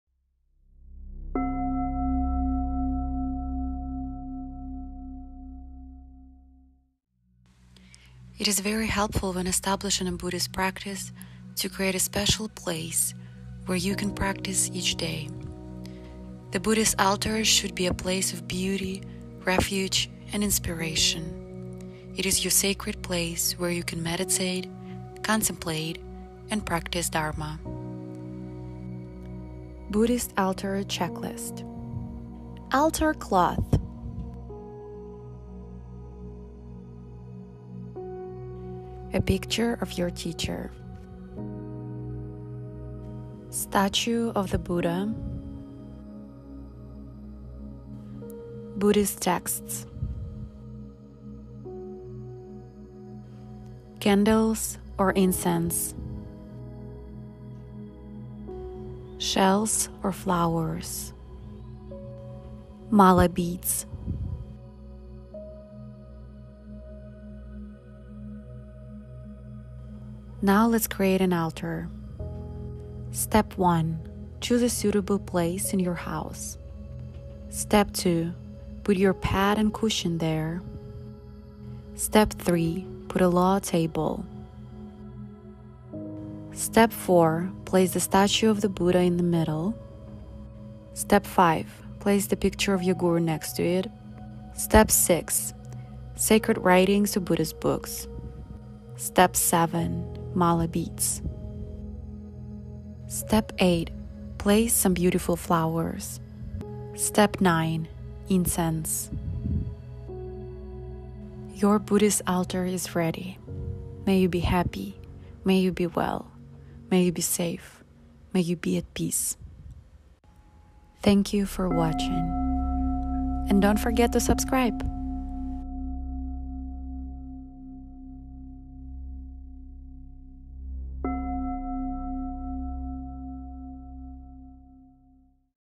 HOW TO CREATE A BUDDHIST ALTAR/ 9 STEPS EASY GUIDE
Home Meditation Altar Checklist:
📿altar cloth, or fabric
📿main sacred item such as a statue or a picture  of a loved one, guru, or teacher
📿a statue stand can help to keep a Buddha statue raised above the other items
📿candles, incense, shells or flowers in offering bowls or incense burner
📿crystals, gemstones, or mala beads

📍Choose a suitable place in your house
📍Put your pad and cushion there
📍Put a low table for incenses, flowers, and a statute (picture) of the Buddha (with your a picture of your Root Guru, Buddhist deities, scared Tibetan writings).
📍The altar should have symbols of the Three Jewels. The Buddha's speech - Dharma - symbolizes the sacred Buddhist text. The body of the Buddha - Sangha, the Buddhist community - is represented by a stupa - a reminder of the Buddha's Enlightenment or a bell with a vajra. The enlightened mind of the Buddha is reminded of a statuette or image of the Buddha Shakyamuni himself. These are required attributes on the altar.
📍Place offerings of flowers, incense, and light on your altar to show gratitude for the blessings in your life and to express your commitment to lead a mindful, compassionate life.